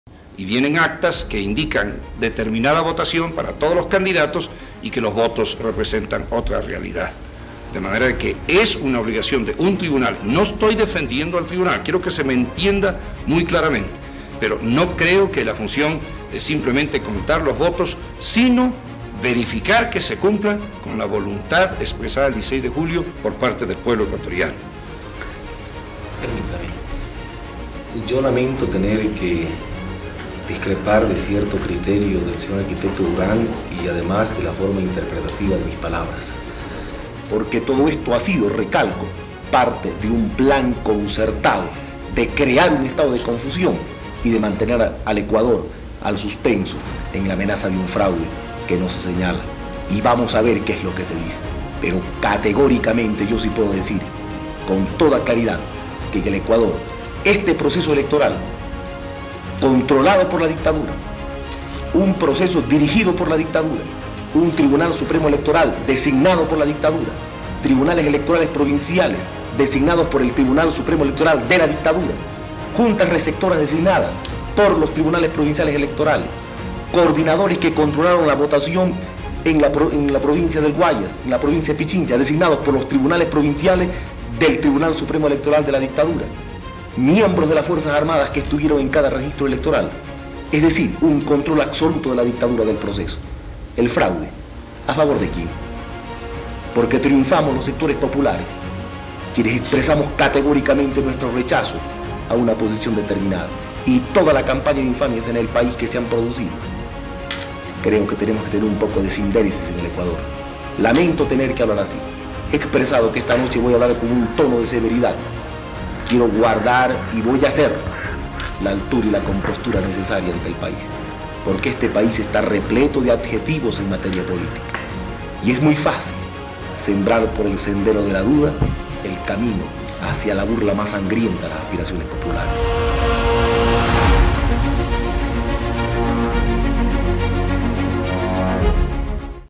1978 Roldós contra Sixto DEBATE
Luego de la primera vuelta electoral, cuando Ecuador retornaba a la democracia, luego de un triunvirato militar. El candidato considerado oficialista, Sixto Durán Ballén, debatió contra el candidato del CFP, quien crecía como la espuma en los estratos populares.